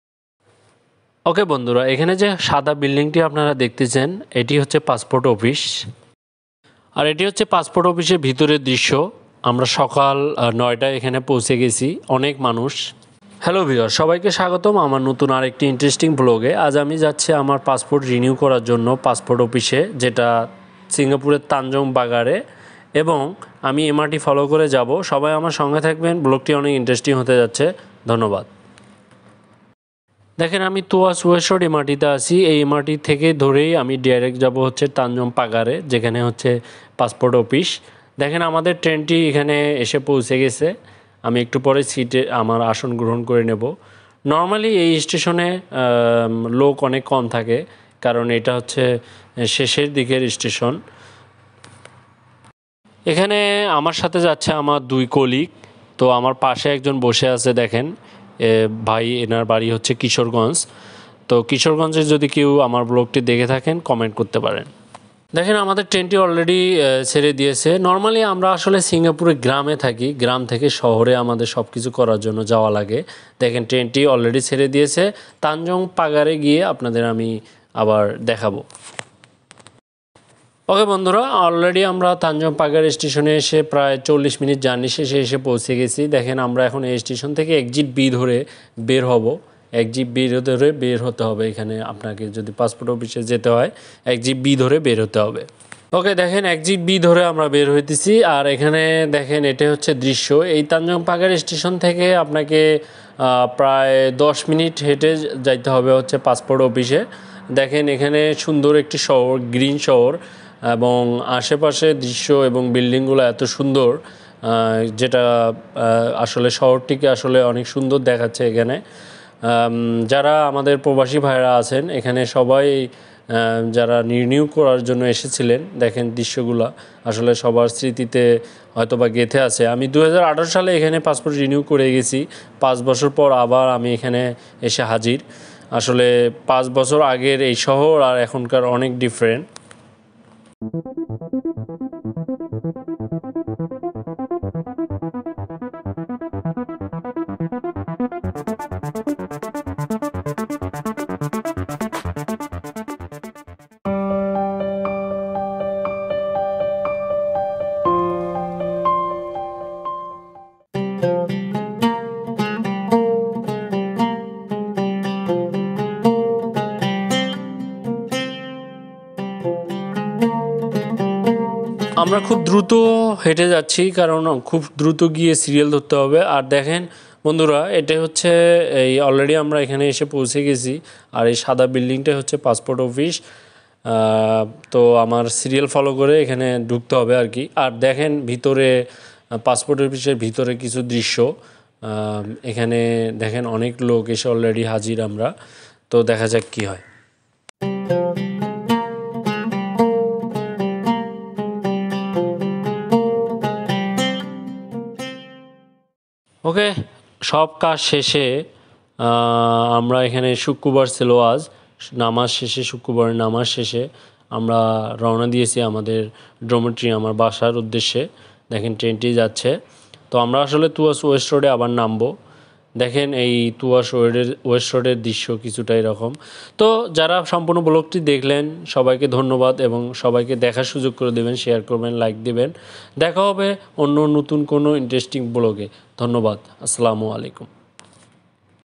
সিঙ্গাপুরেই আমাকে পাসপোর্ট করতে হলো | Passport Office in Singapore
Singapore vlog by sariful
hooe all of you enjoy!